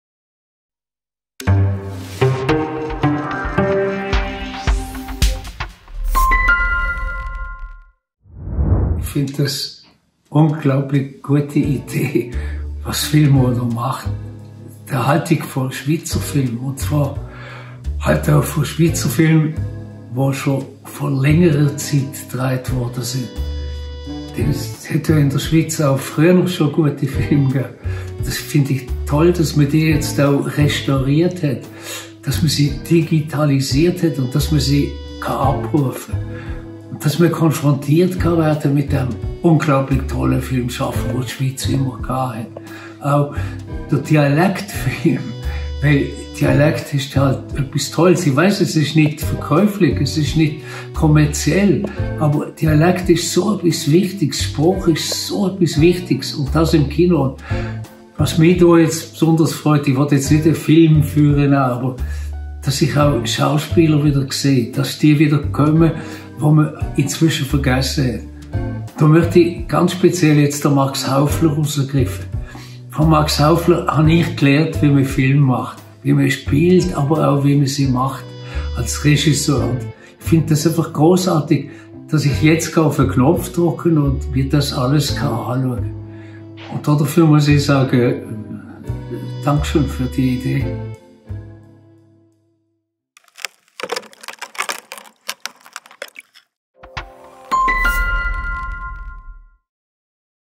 HELMUT FÖRNBACHER | filmo ist für mich... | 2022
Was ist für dich filmo? Wir haben bei Schweizer Filmschaffenden nachgefragt, was filmo für
sie bedeutet. HELMUT FÖRNBACHER findet, es ist eine unglaublich gute Idee, was filmo da macht und sagt Dankeschön.
SOMMERSPROSSEN
GROUNDING
Schweizer Filme neu entdecken
filmo 2022